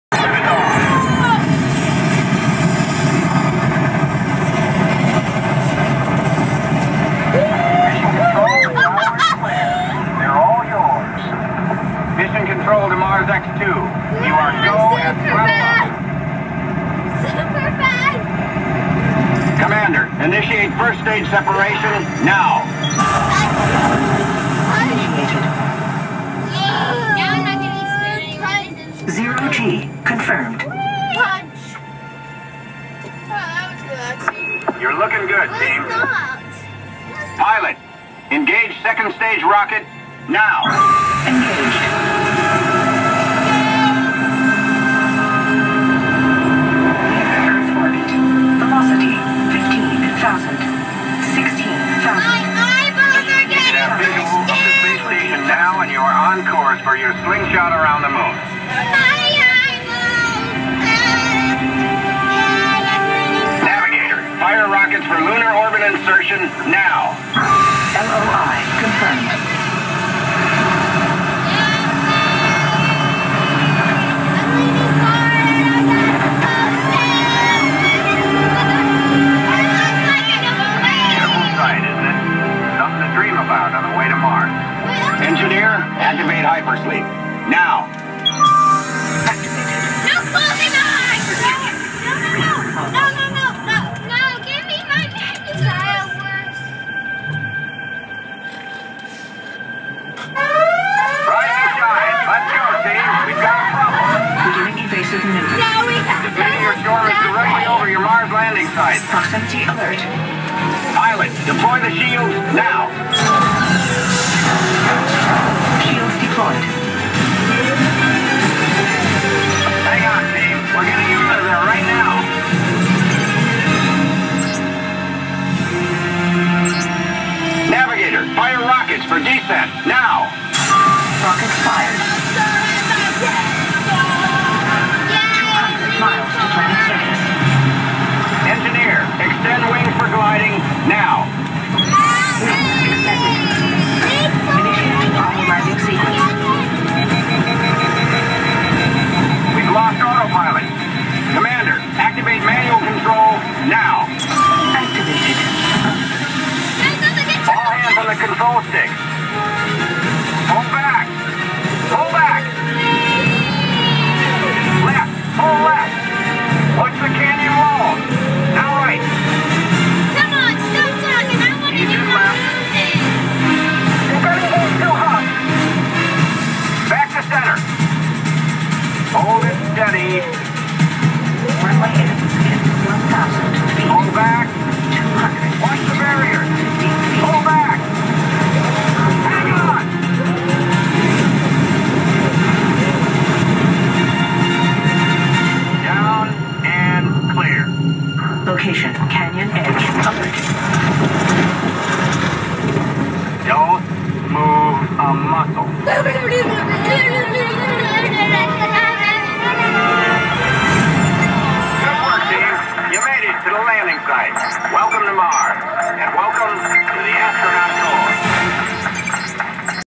2012 10 Mission Space Ride
Siebring kids recorded the Mission Space Ride at Disney's Epcot